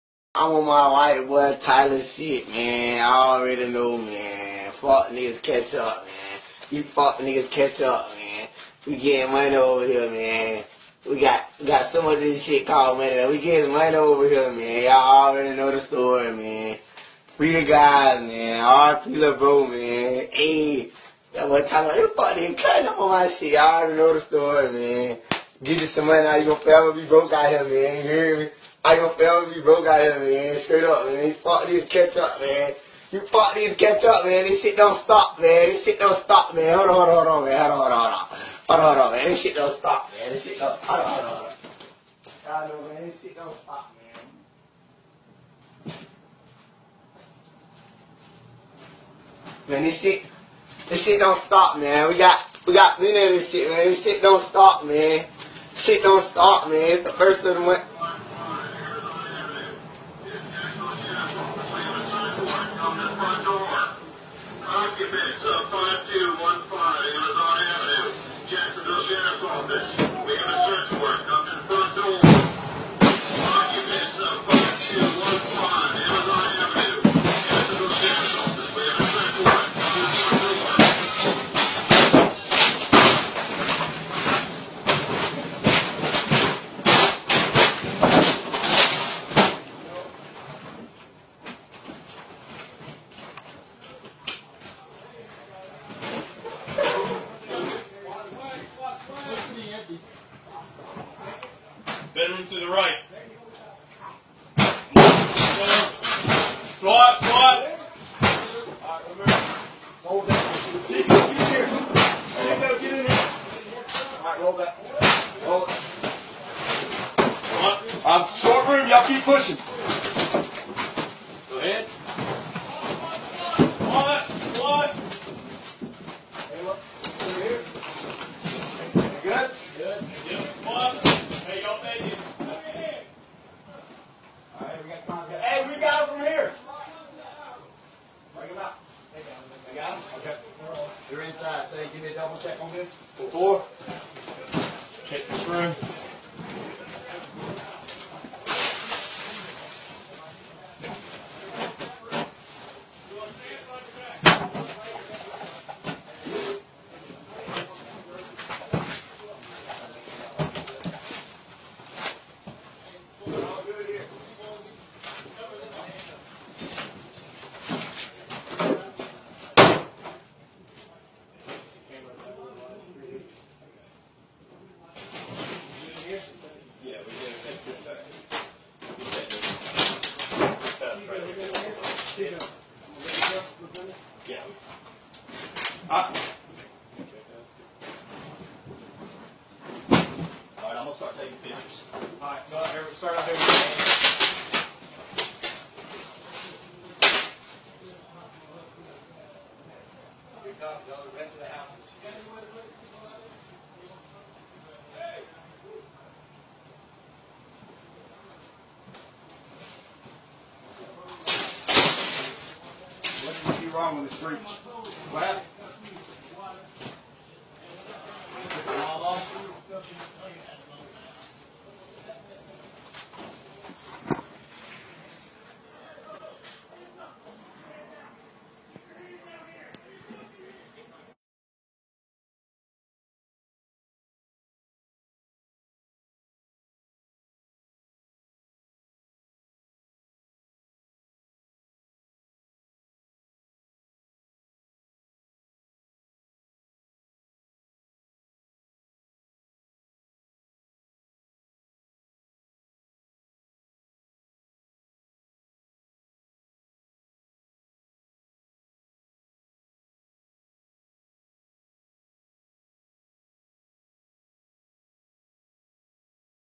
DETENIDO EN VIVO NARCOTRAFICANTE
Un video  muestra el supuesto momento en que la policía de Estados Unidos, allana la casa de un narcotraficante justo en el momento en que este mostraba su dinero obtenido.